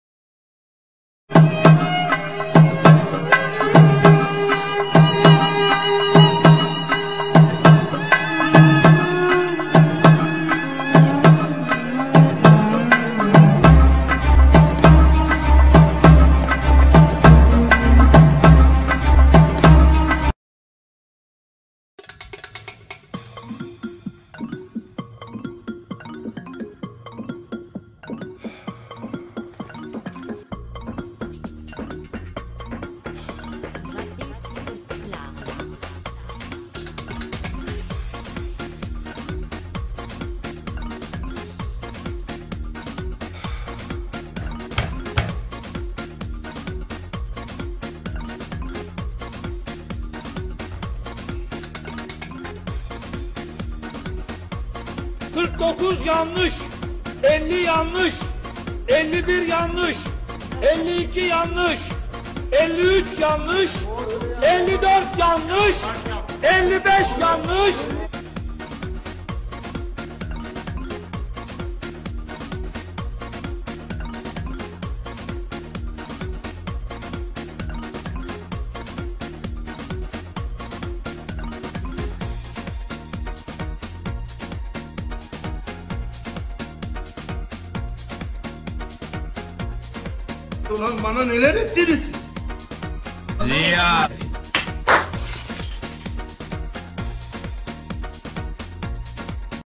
YKS 2021 DEĞERLENDİRİLİYOR / SINAV SONRASI İLK İZLENİMLER
SINAV GÜNÜ YAŞANAN GARİP ŞEYLER:)
AYT sınav sonrası...